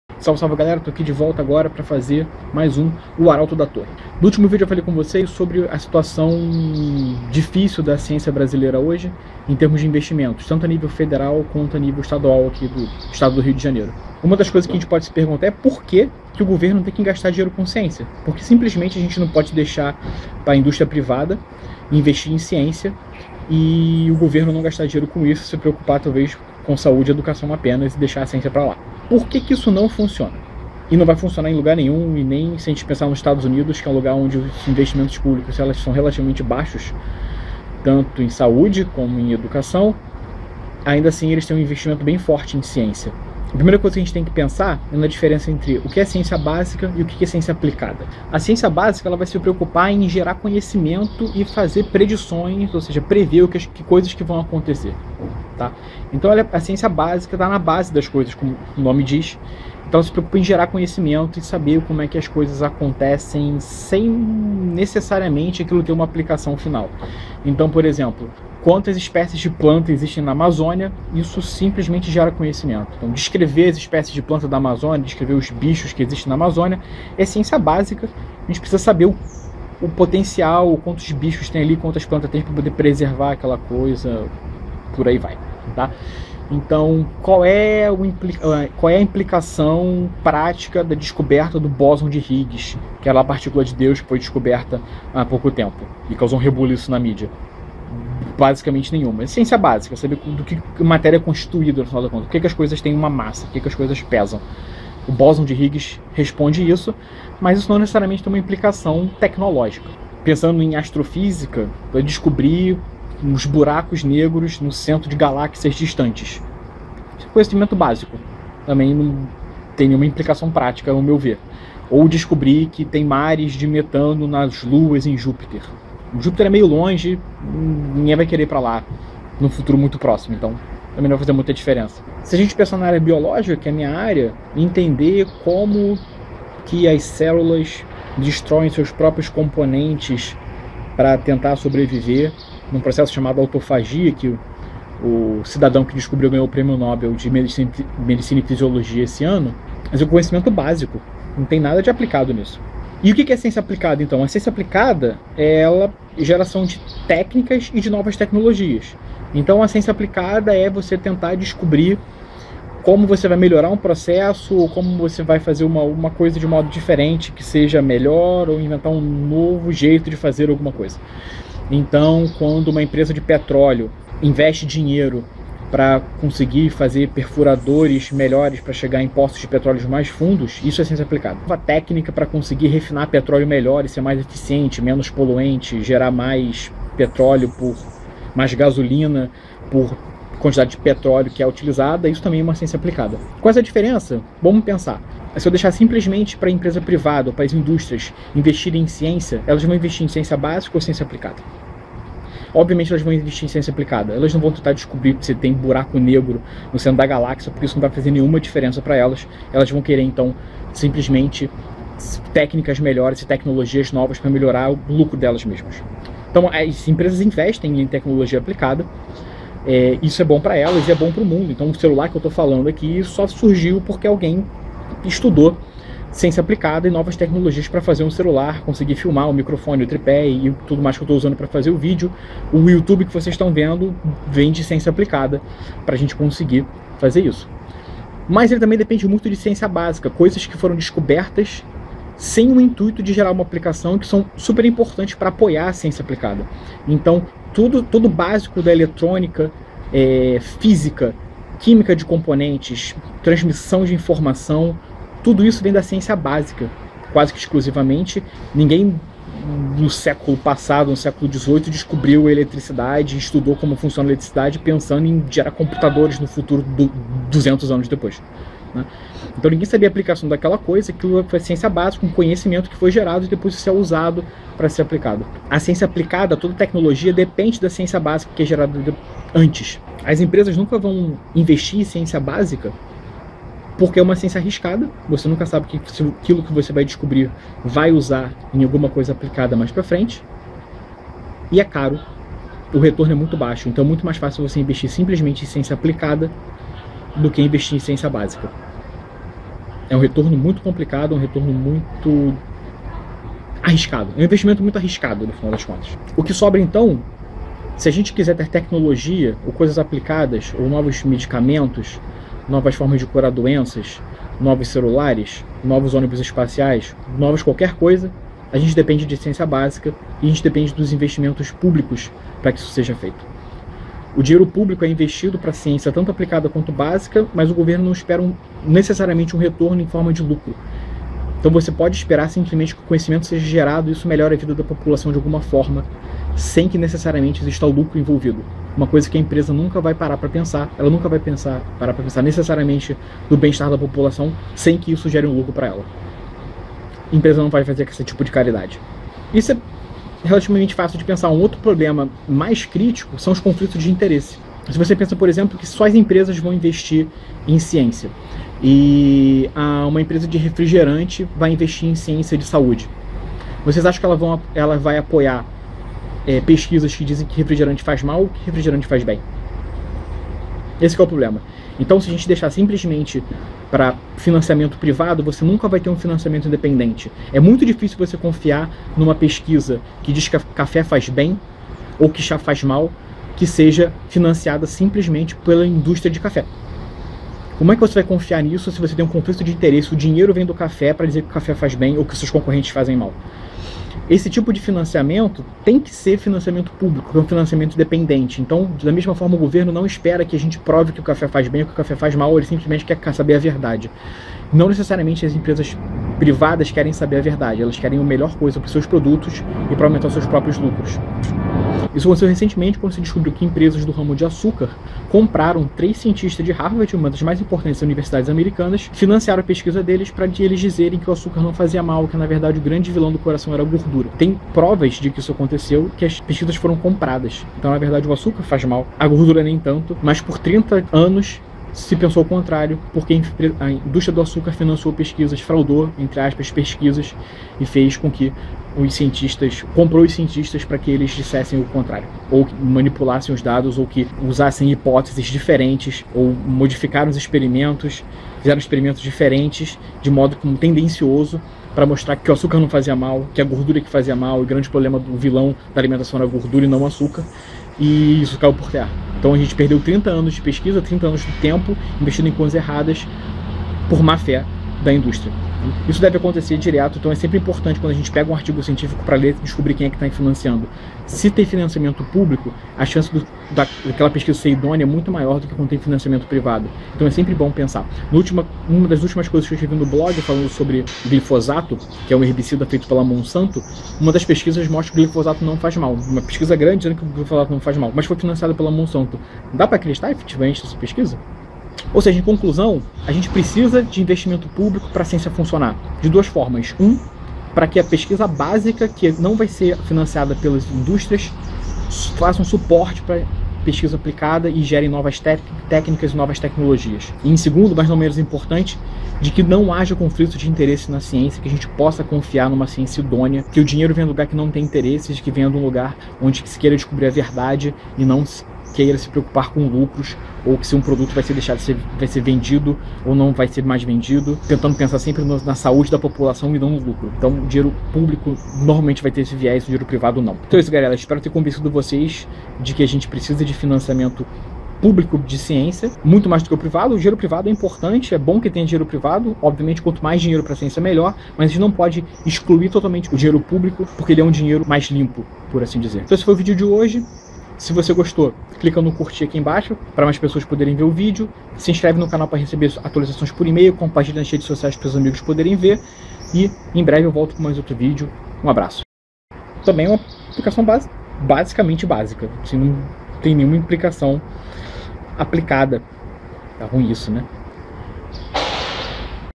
O Arauto da Torre #7: Por que precisamos de dinheiro público na Ciência?
Será que dá para fazer Ciência boa e honesta só com financiamento privado? Assista e confira!

Quer mais? Visite o blog curta a fanpage no Facebook (/aportademarfim) e siga a gente no Twitter (@aportademarfim)!